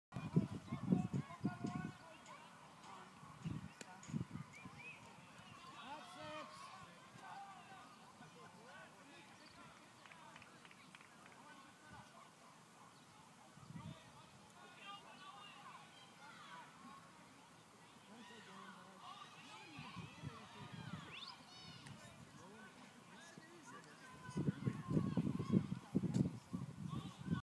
End of the inning
The point of this video isn't the action, it's just really pretty. I want to experiment with interesting angels and composition.